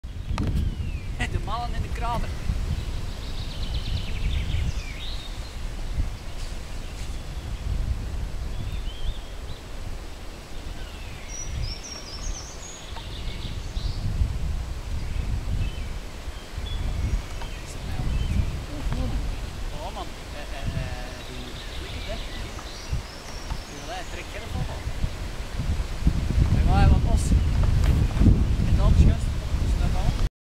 WW2 relic bodemvondsten , Krater na expolsie Duits 15 cm Geel Ten Aard
Krater na Explosie 15 cm Duitse bom te Geel Ten Aard , met dank aan searcher Jef Lievens